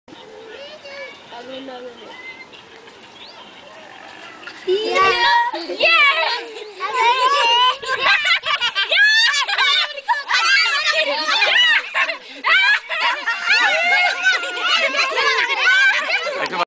Real enthusiasm
A traveller show kids some pictures he has taken with his camera